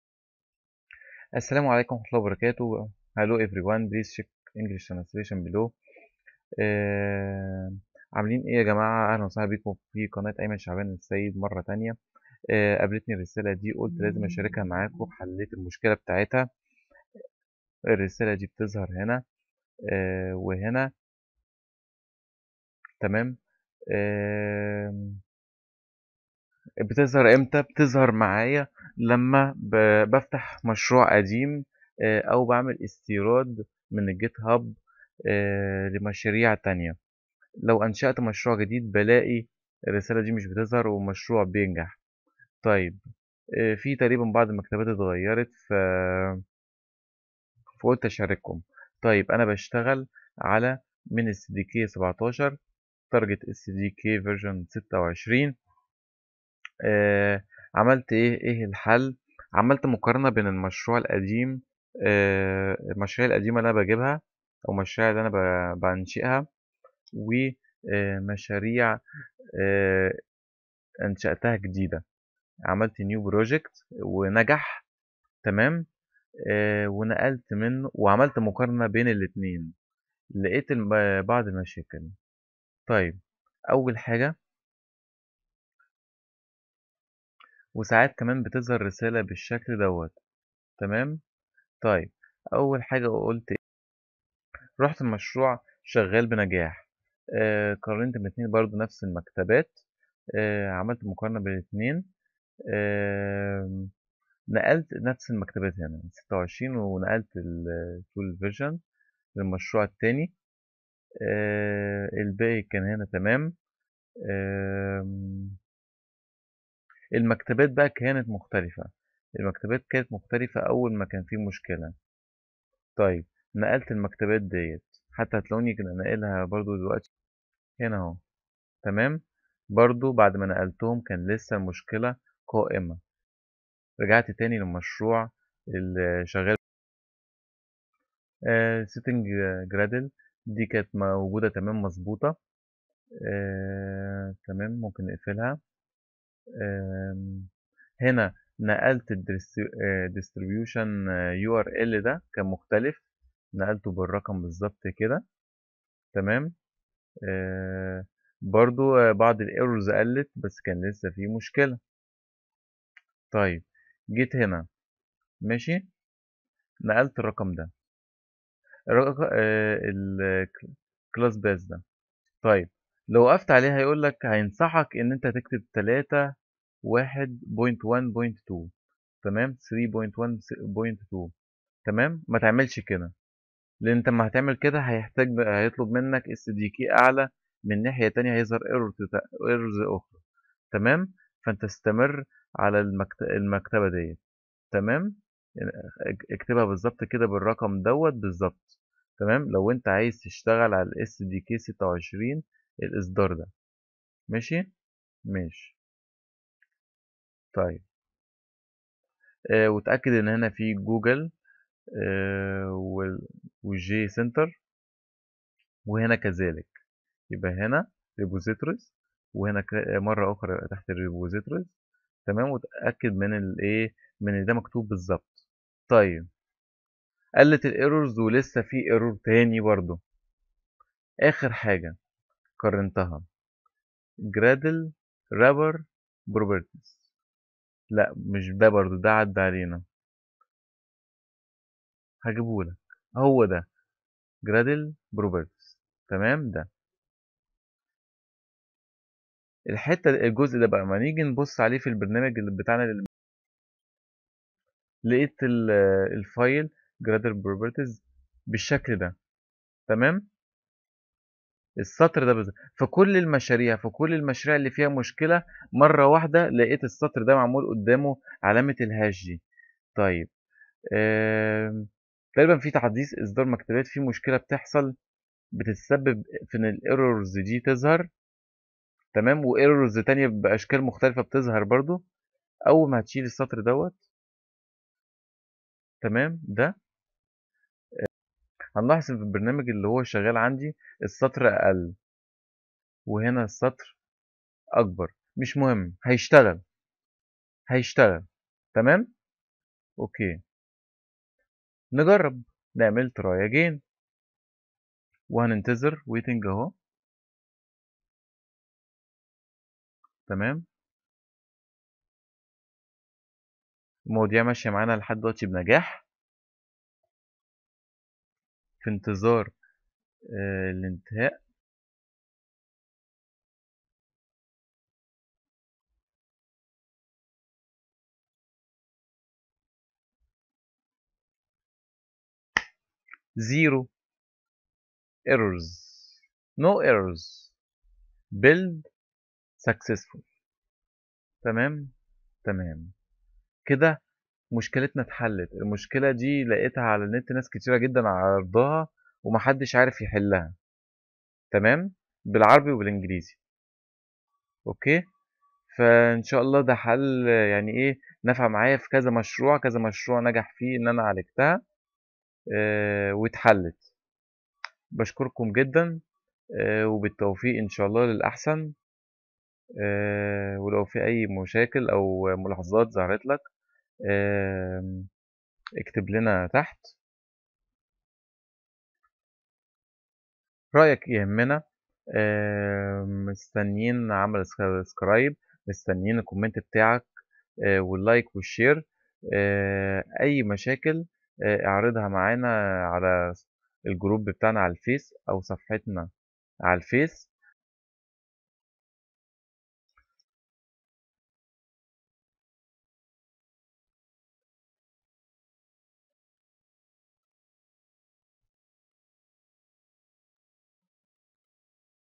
Error Fix : An existing connection was forcibly closed Received , invalid response from the daemon
please cheeck english subtitle
this lesson will be good if you faced one of this errors
سيساعدك هذا الفيديو عندما تواجهك احد هذة الاخطاء
Gradle sync failed: An existing connection was forcibly closed by the remote host
Gradle sync failed: Received invalid response from the daemon : ‘org.gradle.launcher.daemon.protocol.ForwardInput@6c1ec3c7’ is a result of a type we don’t have a strategy to handle. Earlier, ‘Build{id=c2aadf65-5e96-4f9e-abdb-a19c5aab4516.1, request was sent to the daemon.
09:21 م Error running app: Gradle project sync failed. Please fix your project and try again.
gradle project sync failed
gradle synchronization failed
closed by host
diamon android studio
chat: